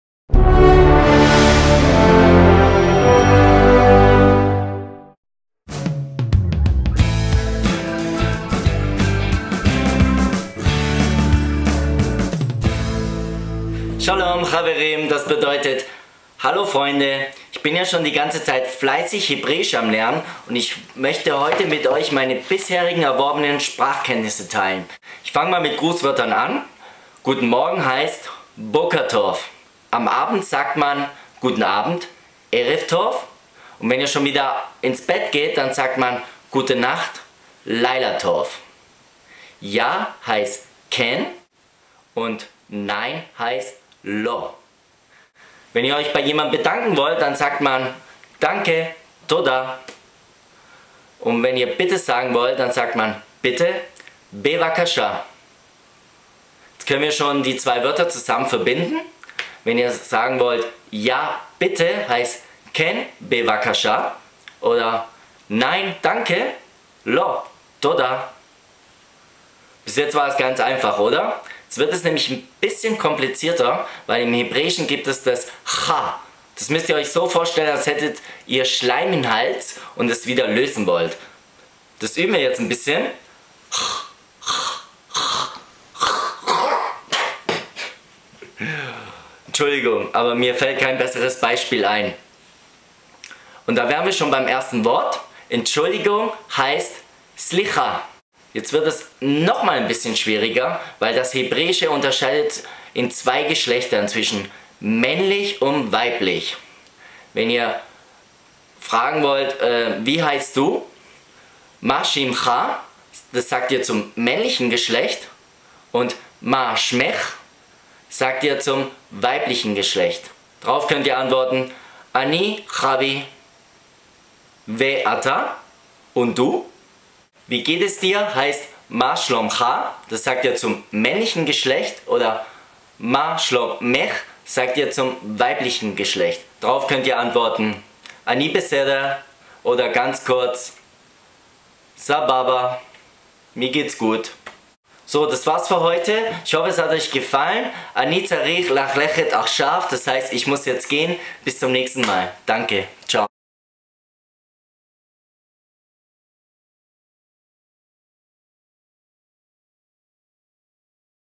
Hebräisch für Anfänger / Hebrew for beginners
Ich bring mir selber hebräisch bei. Was denkst du?  Korriegere mich falls ich ein Fehler gemacht habe.

I am learning hebrew by myself, please tell me what you think, and correct me if i made mistakes.

Freue mich über jeden Kommentar.

Hier die Playlist wenn ihr mehr hebräische Videos haben wollt.